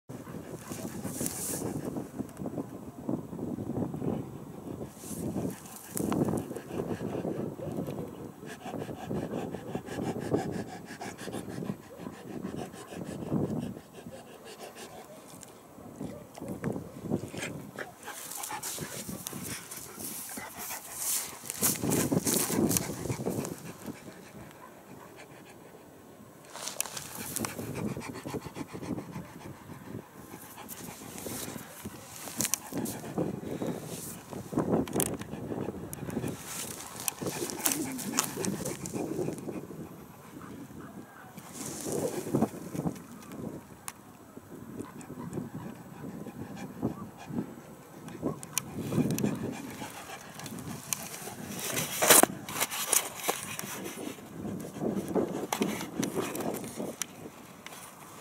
My Dog Caucasian Ovcharka
I went out for a walk with the dog caucasian ovcharka on the hill